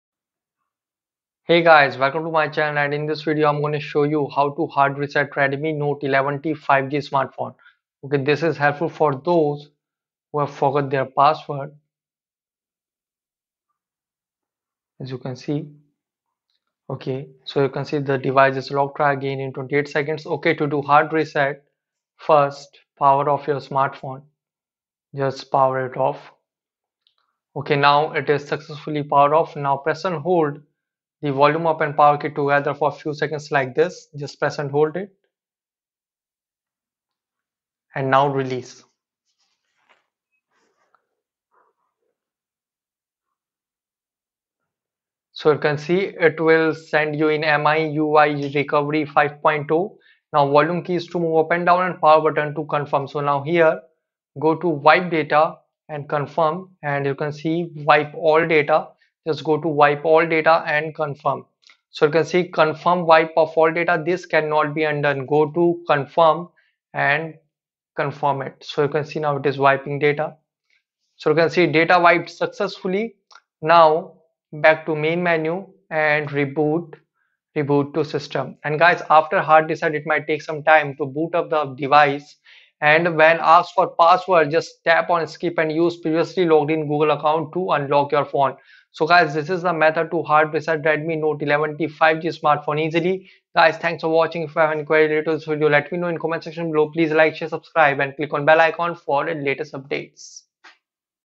How to Hard Reset Redmi Note 11T 5G - Forgotten Password/Factory Reset
How to Hard Reset Redmi Note 11T 5G Smartphone. If you forgot the security password of your Redmi Note 11T 5G. This will work for Note 11T 5G and any other Redmi Smartphone. [Factory reset/wipe all data]